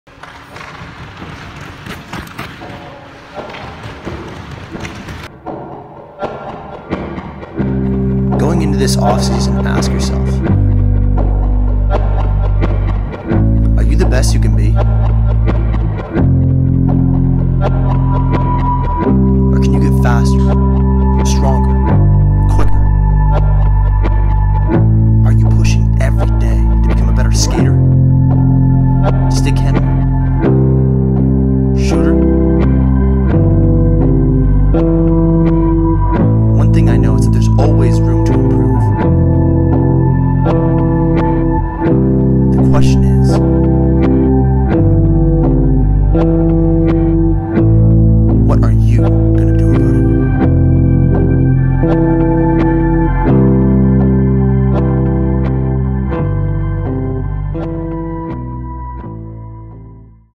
Train Insane. Or Remain the Same.
This is a short film / advertisement piece that I have been working on for the Rochester Ice Center Hockey Academy.

Moral of the story is that you get out what you put in.
If the work is put in you can make your biggest leaps during the off season.

Motivational Monday